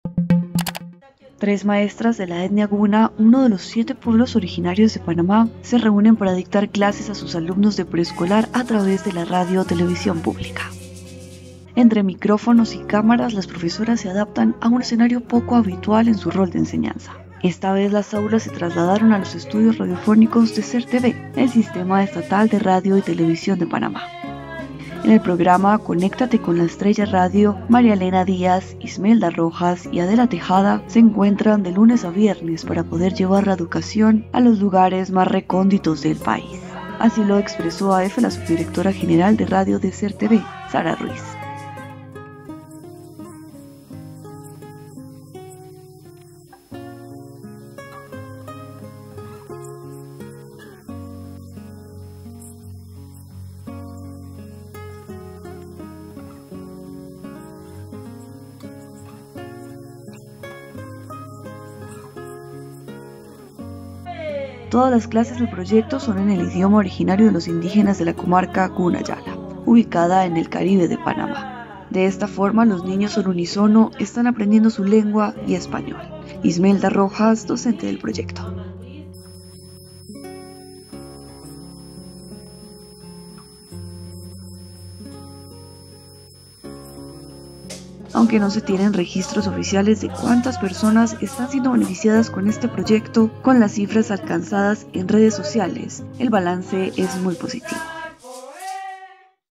Panamá imparte clases a niños indígenas en su idioma en la radio
"Doggus nued we wagdaradba doddogan anmar iddosimalad", enfatizan a la vez en su idioma ancestral las tres maestras de la etnia guna de Panamá, vestidas con su colorida mola para dar los buenos días a sus alumnos y arrancar su clase de preescolar en la radio pública.

Son las ocho de la mañana de un viernes y "le toca" a los estudiantes de cuatro y cinco años. Pero los más pequeños no están presentes: las docentes están en uno de los estudios radiofónicos de SERTV, el Sistema Estatal de Radio y Televisión de Panamá, bajo una situación poco habitual para ellas, quienes se acomodaron a los micrófonos, cámaras y focos.

¡Visítenos!